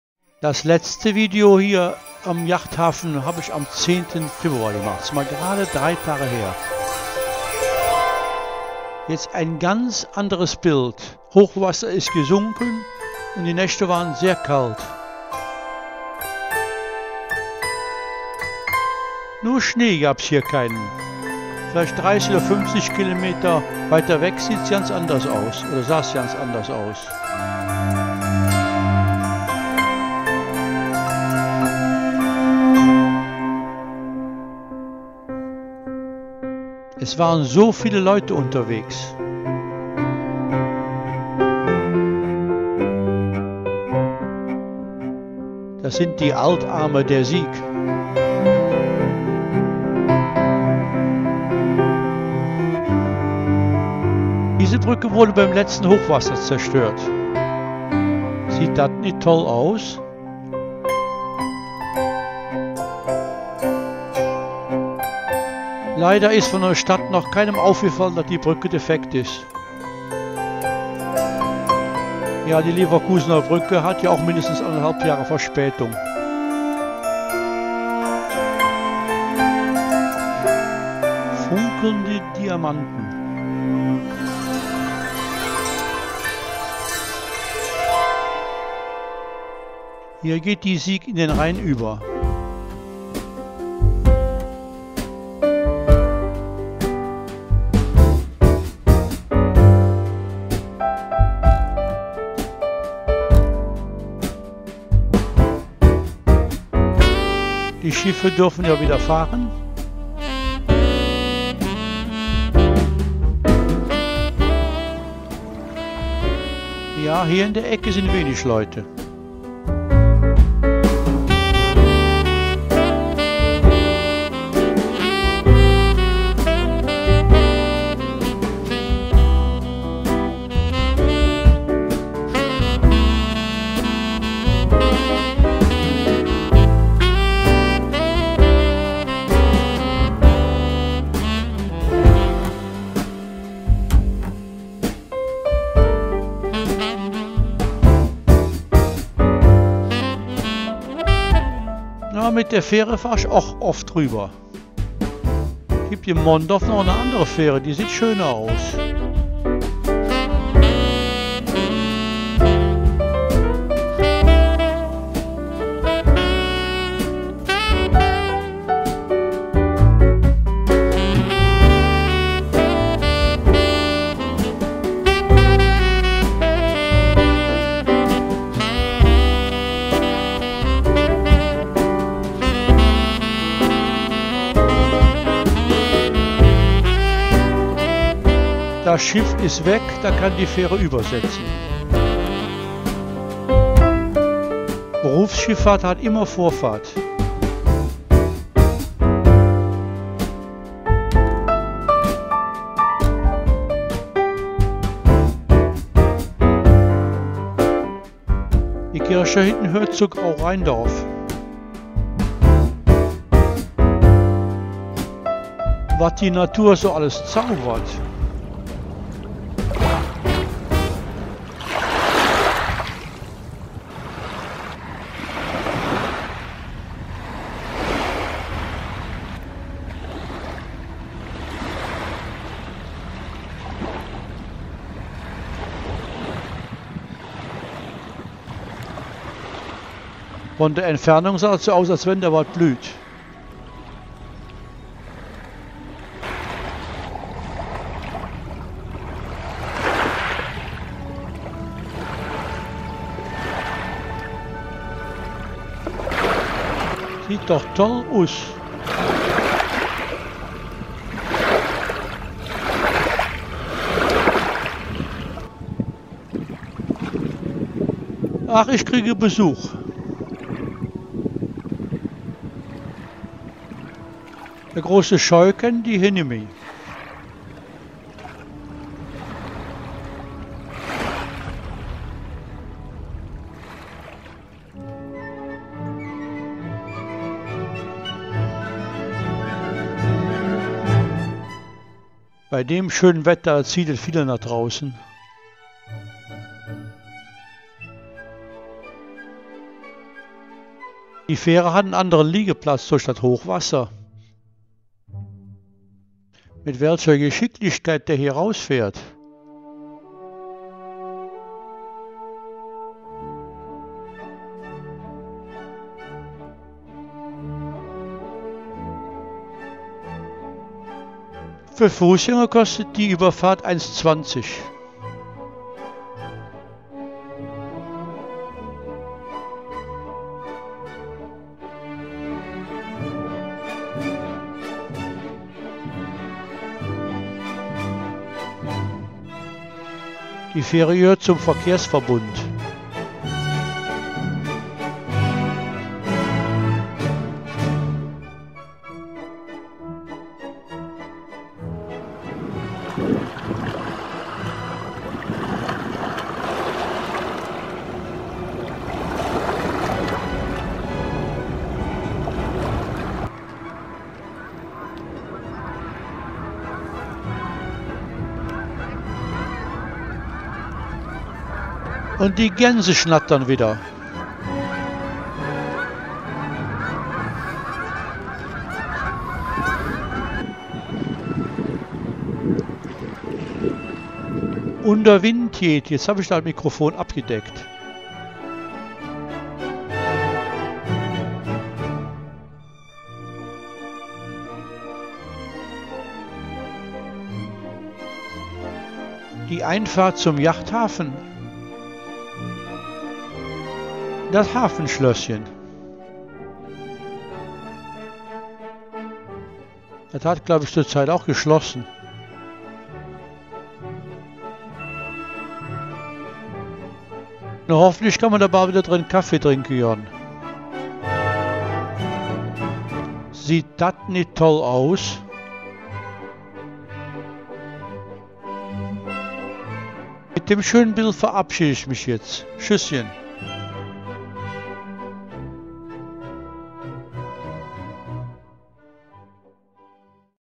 Mondorf/ Rhein 13. Februar 2021
Es zieht mich zum Vater Rhein etwa 100 Meter dann bin schon
am Hafen. Ist das kein Kaiser Wetter? Wie von Diamanten
übersät funkelt der Rhein.